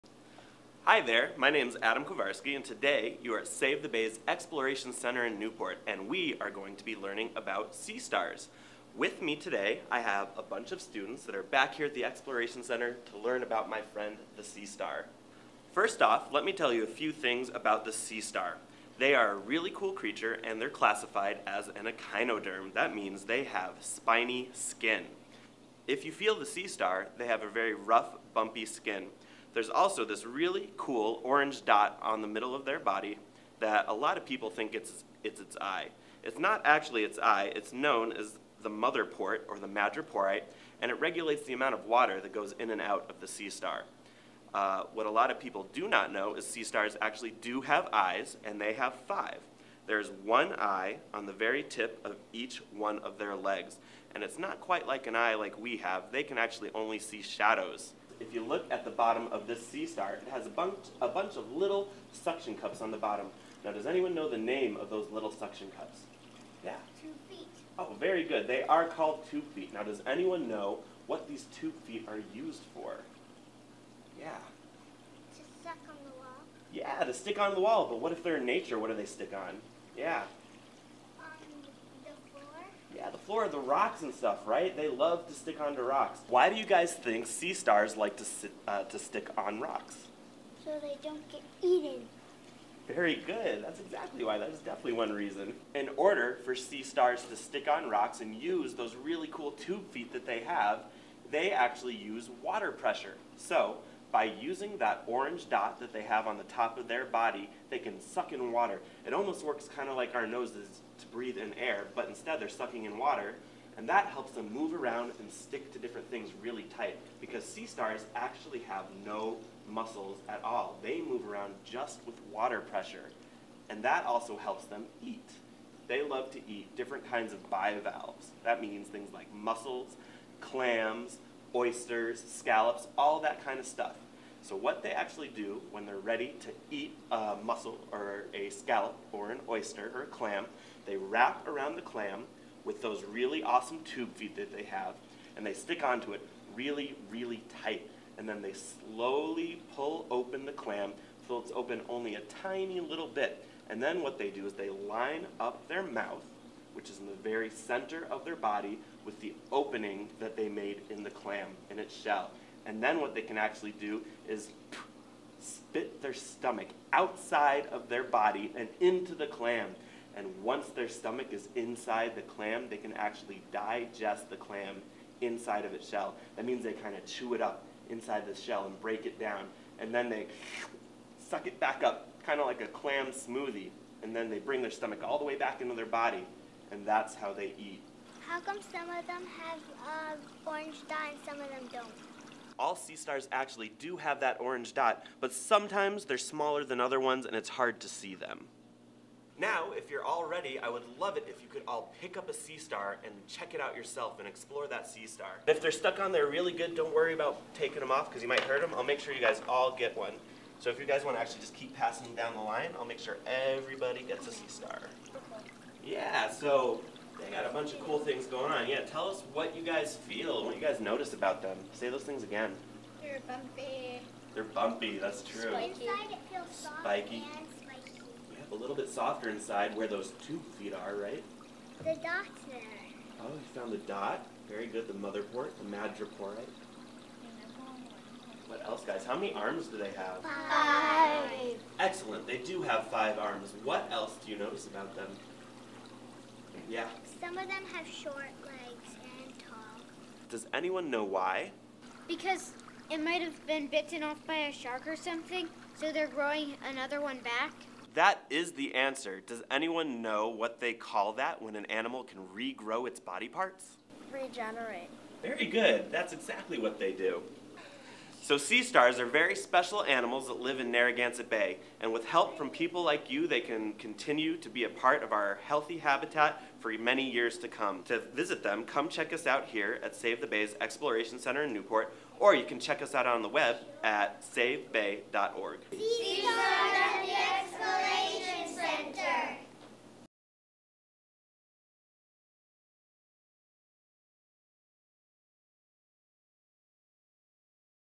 EXPLORE: Sea Stars
Save The Bay educator Adam Korvasky takes a look at the sea star -- one of the many Narragansett Bay marine species on display at the Save The Bay Exploration Center in Newport, RI.